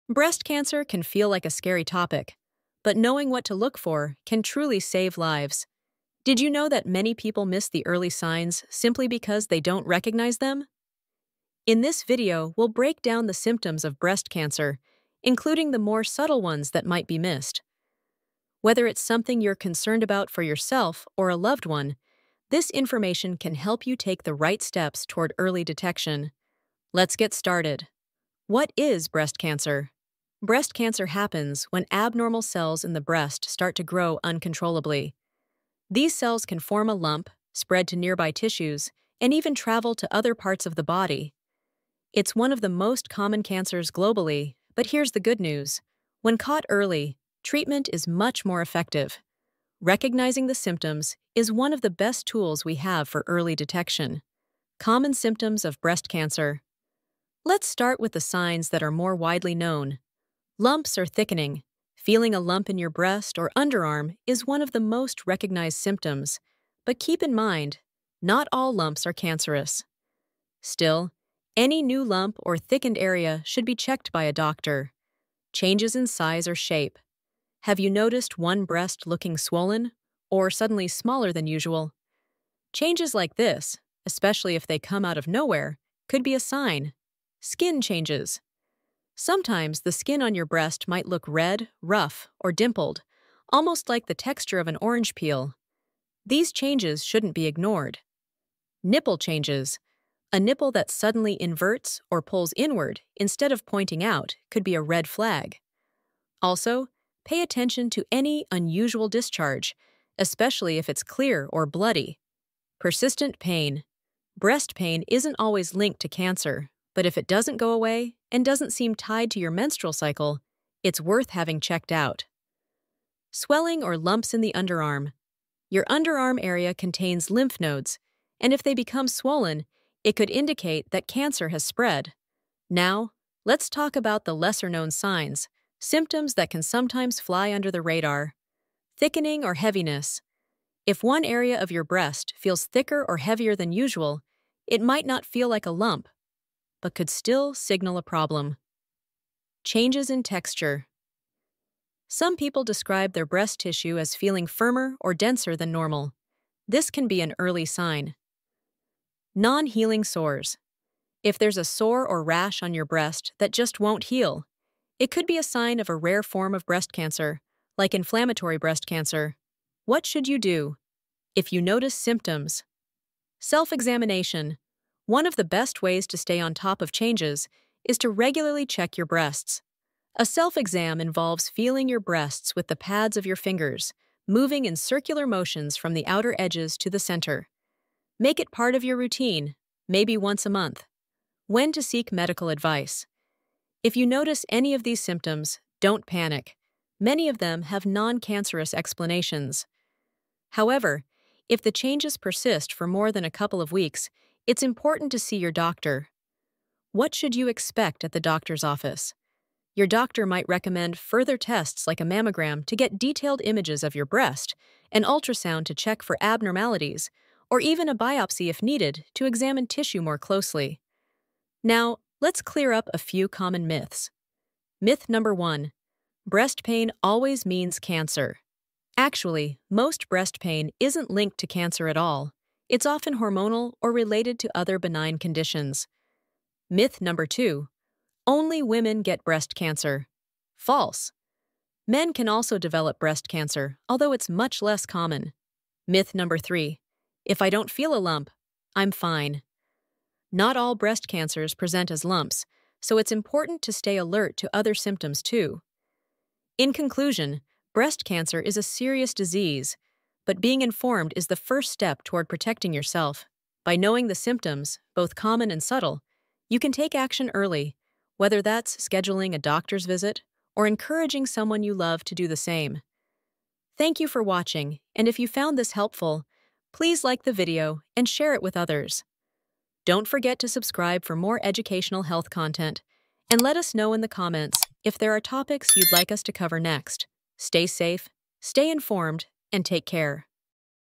Breast Cancer Doesn't Always Look Like You Think
Breast Cancer Symptoms: The Silent Signs You Should Never Ignore 🚨
Early detection saves lives! In this video, we uncover the subtle signs of breast cancer that are often overlooked. From unusual skin changes to persistent pain, knowing what to watch for can make all the difference. Whether you're performing self-exams or just staying informed, these insights could protect your health or help someone you love. 💗

🔍 What You'll Learn:

The top silent symptoms of breast cancer
How to perform a proper self-breast exam
When to consult a healthcare professional

💬 Let's Talk:
Have questions or a story to share? Drop them in the comments below!